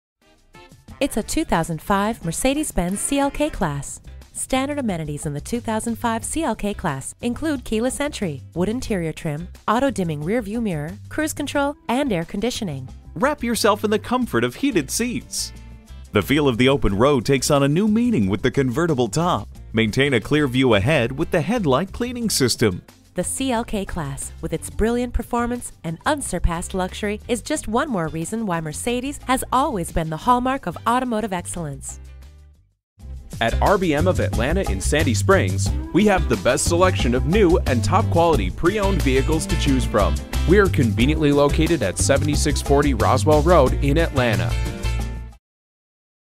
Used 2005 Mercedes-Benz CLK-Class Atlanta GA Sandy Springs, GA L5101PA - SOLD
Year: 2005
Make: Mercedes-Benz
Model: CLK-Class
Trim: 3.2L
Engine: 6 Cyl. 3.2L
Transmission: Automatic
Color: Brilliant Silver Metallic
Interior: Charcoal
Mileage: 73704
Stock #: L5101PA
VIN: WDBTK65G35T055689
CLEAN CARFAX! OPTIONS INCLUDE DVD COMAND NAVIGATION, 6-DISC CD CHANGER, AM/FM STEREO, harman/kardon SOUND SYSTEM, POWER DRIVER SEAT AND STEERING COLUMN WITH MEMORY, AUTO-DIMMING MIRRORS, POWER PASSENGER SEAT WITH MEMORY, HEATED FRONT SEATS, MULTIFUNCTION STEERING WHEEL, BLUETOOTH HANDS-FREE SYSTEM, 17 5-SPOKE ALLOY WHEELS AND BURL WALNUT WOOD TRIM.

Give us a call at:

Address:
7640 Roswell Rd